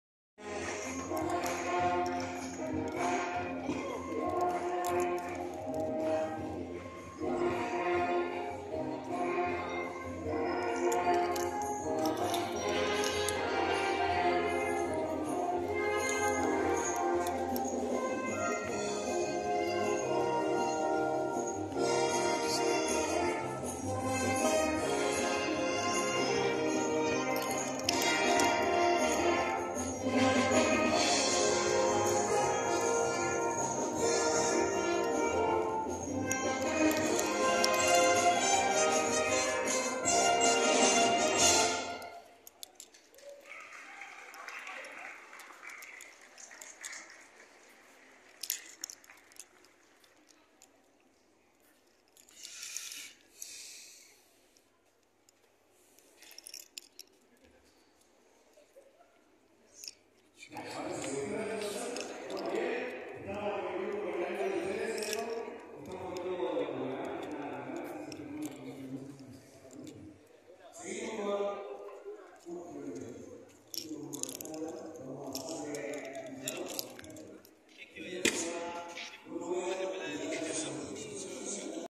Mercedes Benz 300 E plomo en Santiago Chile
Santiago de Chile Santiago Centro Museo Militar Día de los Patrimonios 25 de Mayo 2024